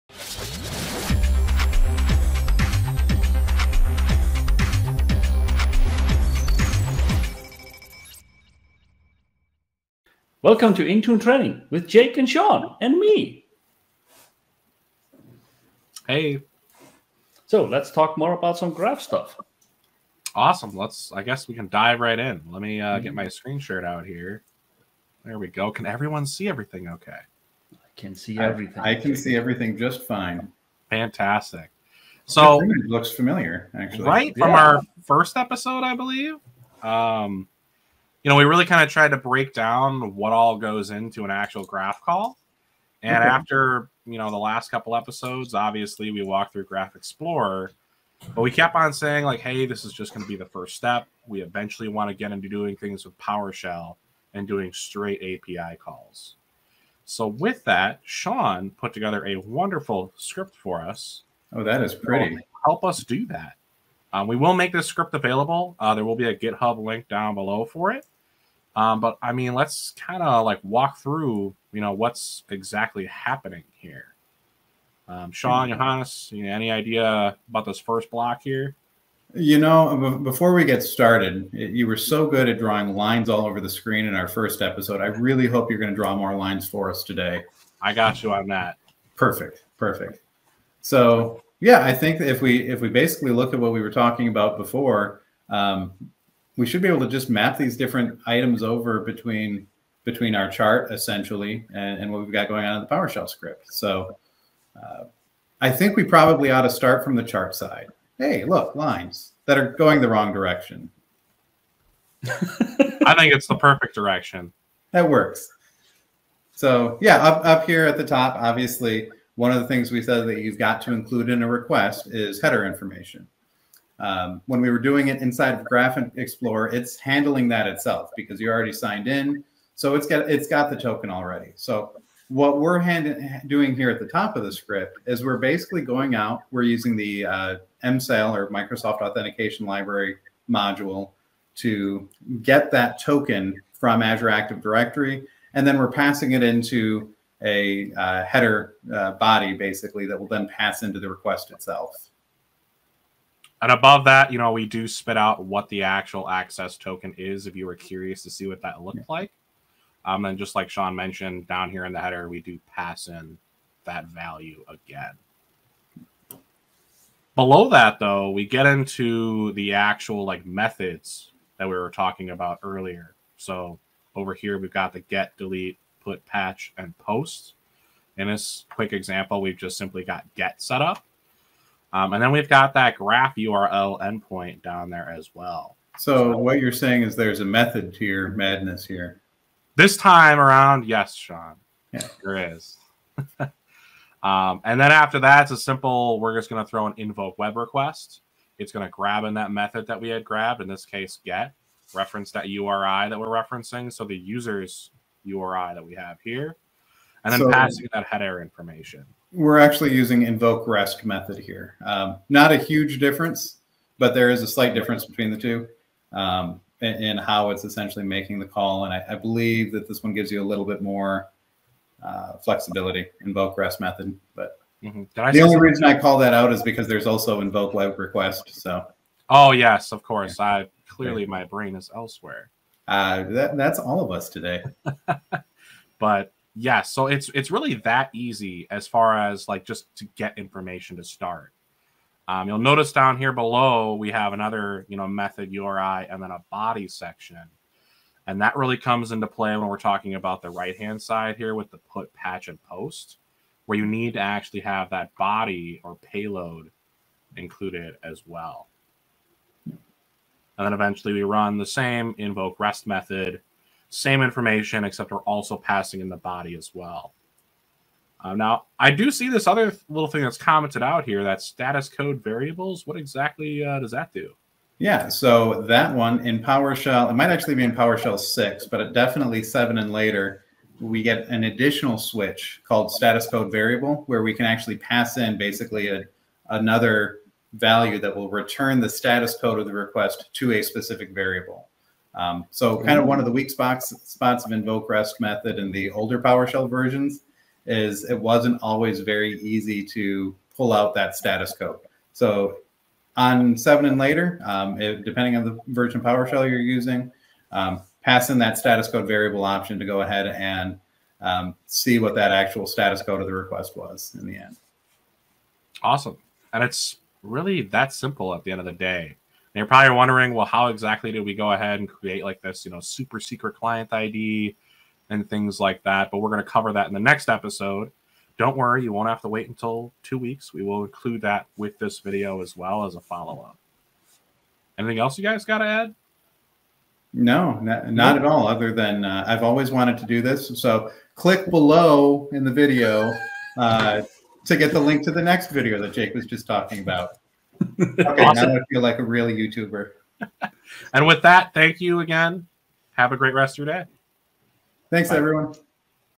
Graph 101 - E5 - Graph 101 Starter Script (I.T)
00:00 - Intro
00:51 - Script download
01:59 - Header information
02:47 - Methods
03:14 - Invoke-RestMethod
04:29 - StatusCodeVariable
05:15 - Wrap up

Apologies next episode will be out next week! Blame Jake!

Visit our websites and social media for more or to get in touch with us